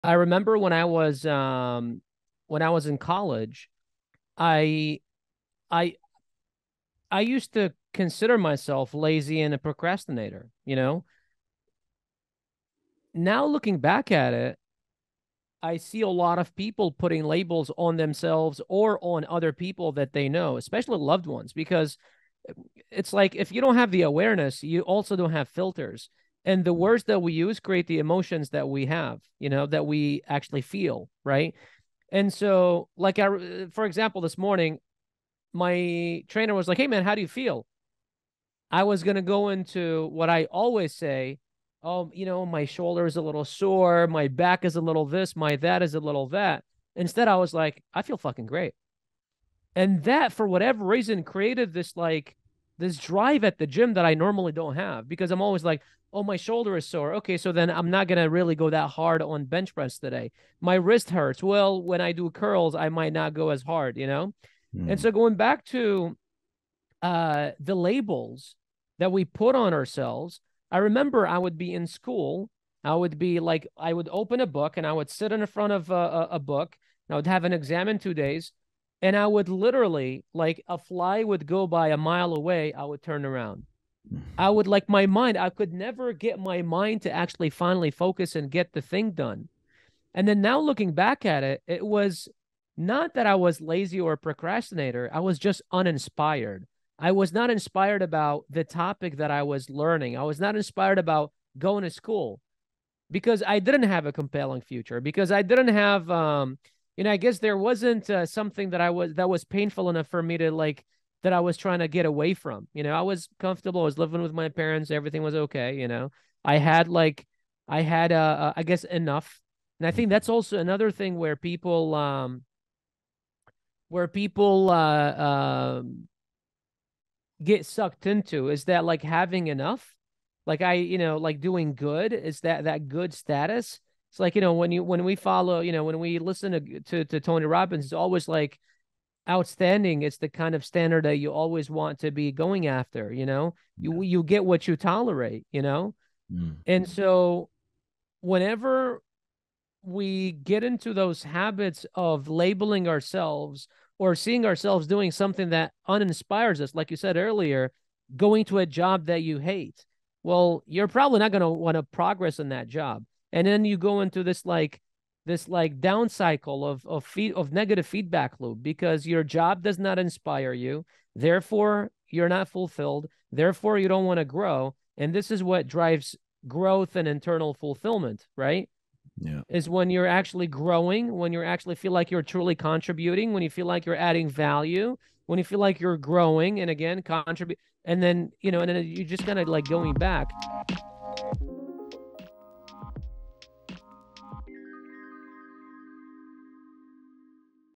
The REAL reason why you're easily distracted
Do you find it hard to focus on your goals? The REAL reason why you're easily distracted may surprise you. Here’s how you can break free from the negative loop that keeps you unfocused

In this video, we'll uncover the root cause of distraction, why the labels you call yourself can have a profound impact on your life and how to overcome laziness and procrastination.

WE WANT TO HEAR FROM YOU:
Comment your questions below and let us know what you want us to cover in the next video.